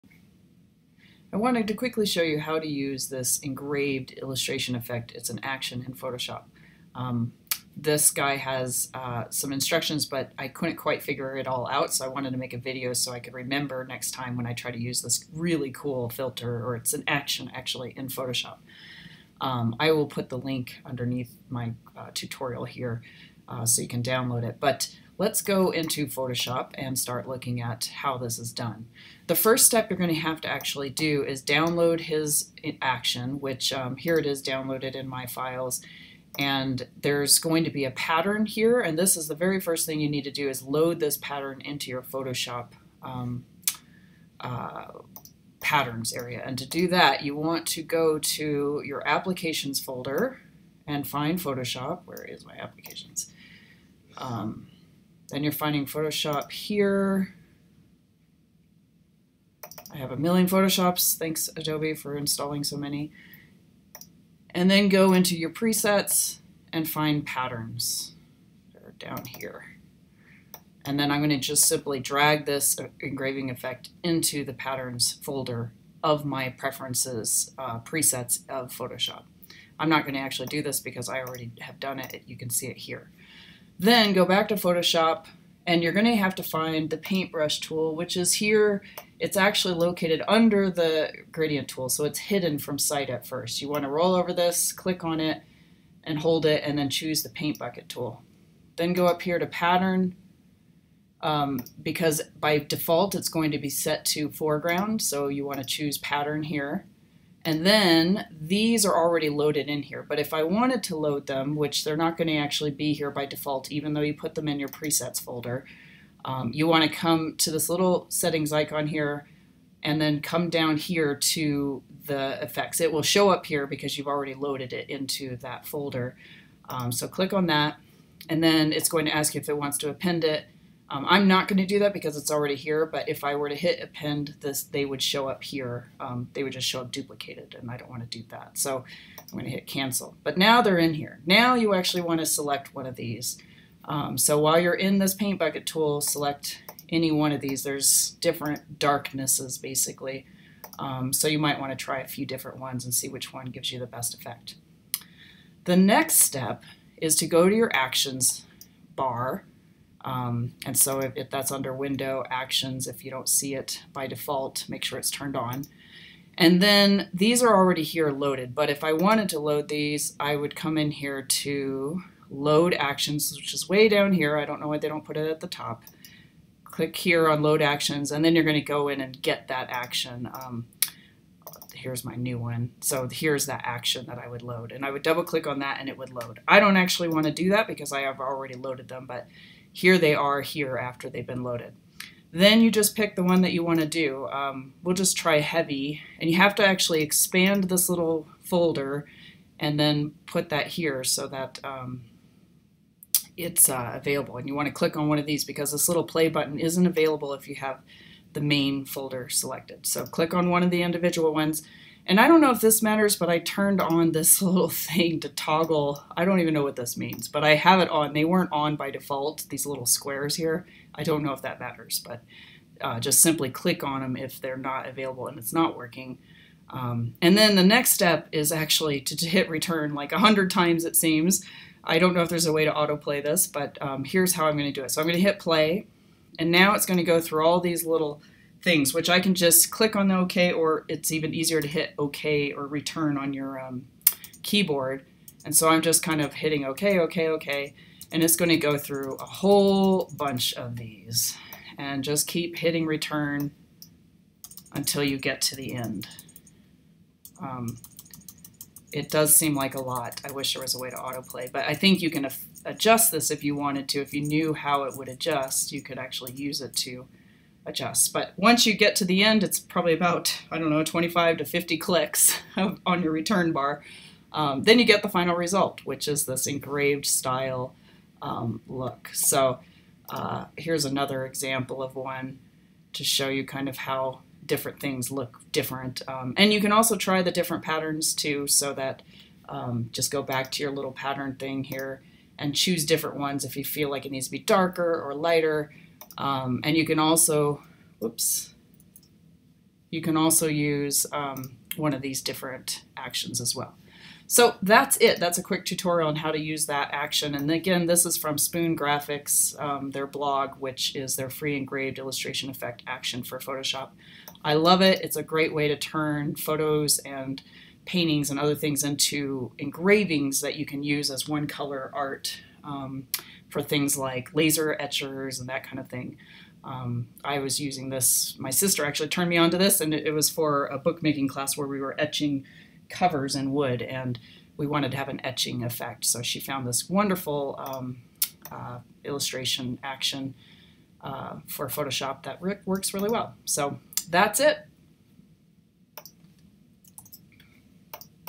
Engraving Photoshop Effect with Actions
How to create an Engraving Photoshop Effect with Actions - here are the details on how to load the pattern, and the actions and use them to create really cool engravings from photos and paintings.